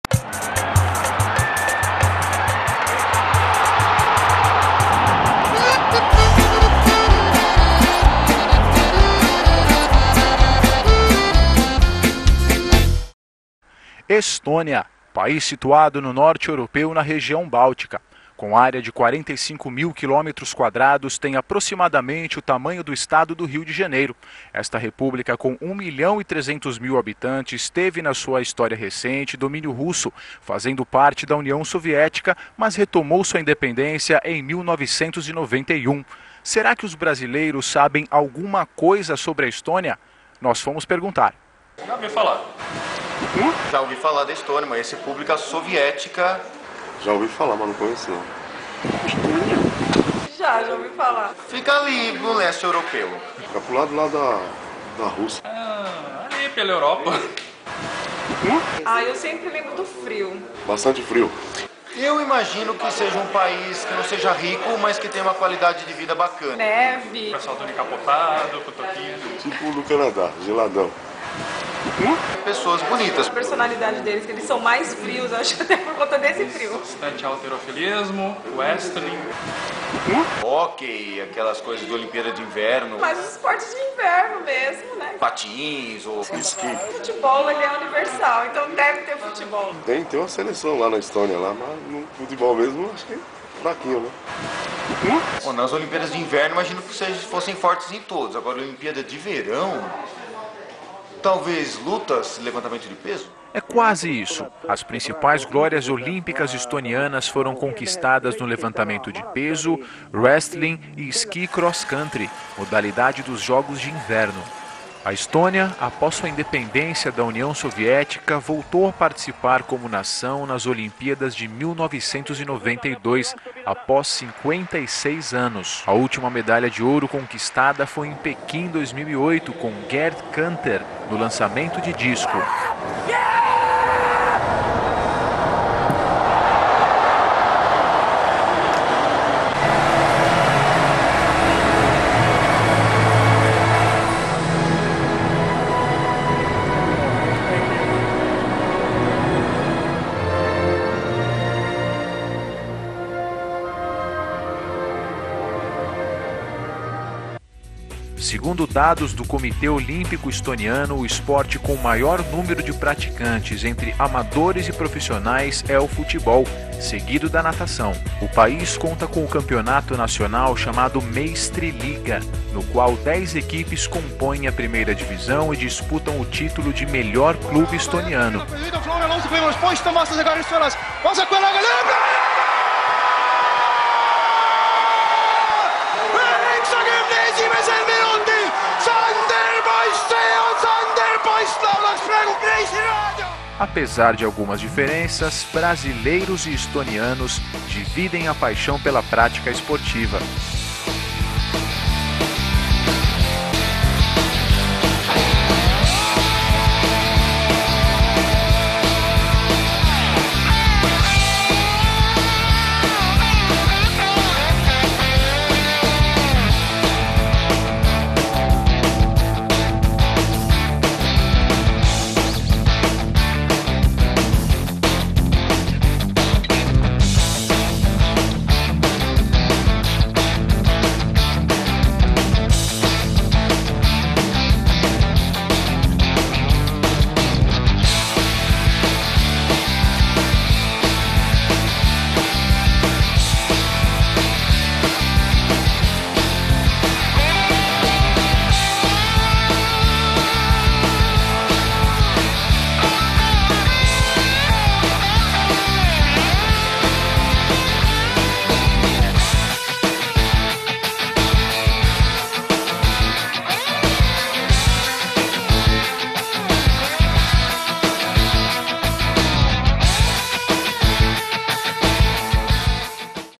Do brazilian people know anything about Estonia? (estonian subtitles)
Milliseid spordialasid harrastavad eestlased?

Küsisime seda brasiillastelt. Siin on nende vastused ja väike kokkuvõte spordist Eestis.

Eesti subtiitritega versioon

"What kind of sports are practiced in Estonia?"

We asked this question to brazilian people. Here you can see their answers and an little overview about sports in Estonia.

Estonian subtitled version